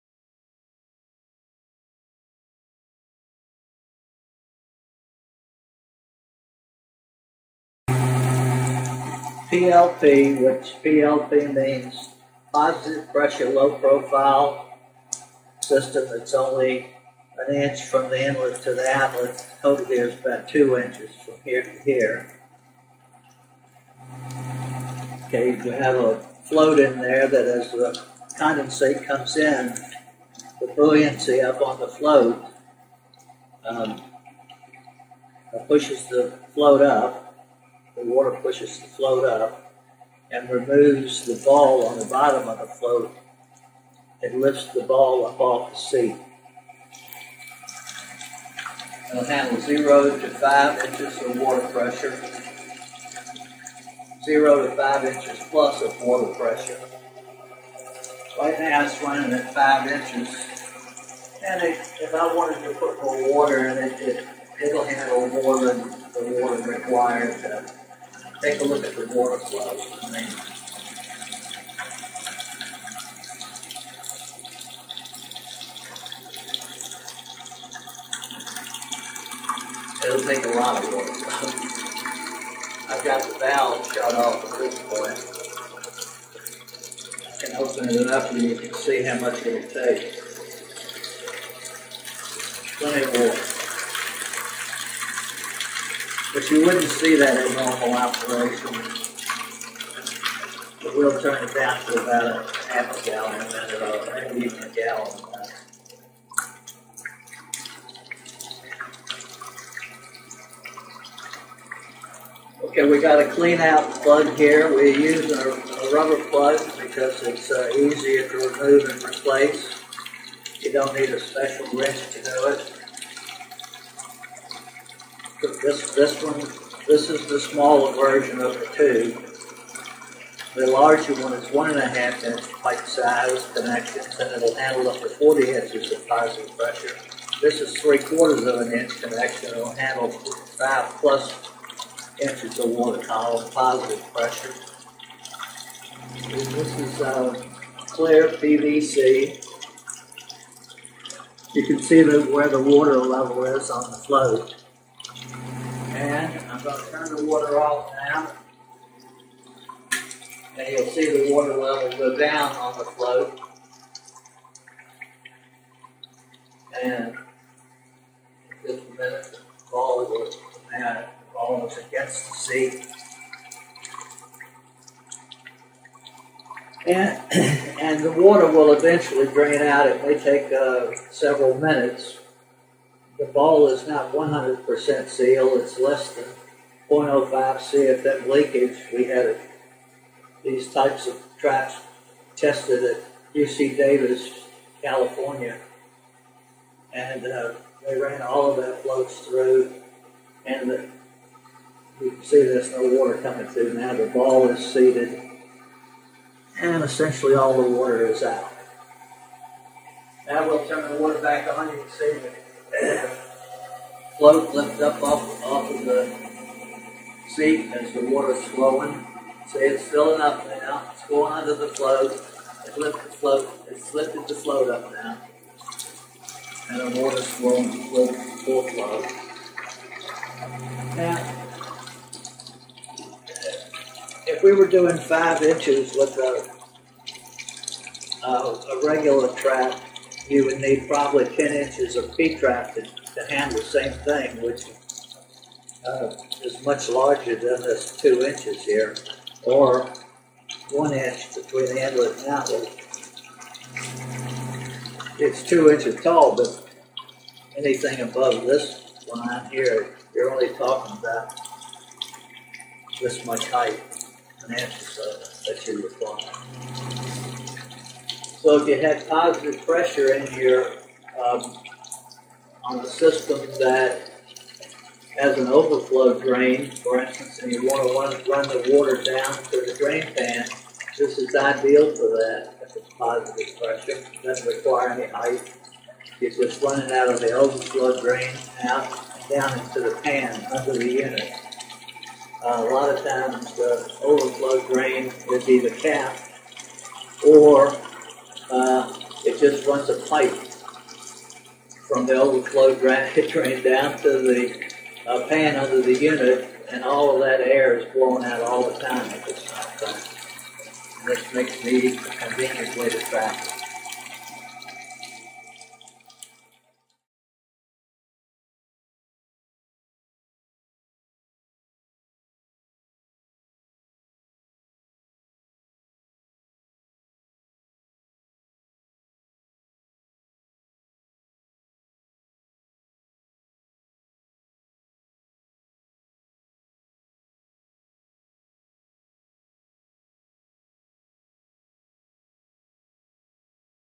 AHR Innovation IAQ Award winner – PLP - | Smaller size, 3/4” is now available!!
Have you heard of the Air-Trap and latest model PLP - Positive Pressure Low-Profile? Check out the video and learn all about the PLP.  The new concept in condensate trap design permits condensate removal from AC or other condensing equipment while blocking airflow all year. Comes in two connection sizes 3/4" and 1 1/2". The 3/4" PLP, one height for all pressures, 2" from inlet to bottom and positive 0-5" WC pressure. The 1 1/2" PLP one height for all pressures, 2 7/8” for positive 0-40” WC pressure.

• Never freezes, never experiences “dry-out”
• Meets IMC® Code Section M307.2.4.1

• Eliminates sludge build-up & air leakage

• Pre-designed to eliminate errors caused by field designed P-traps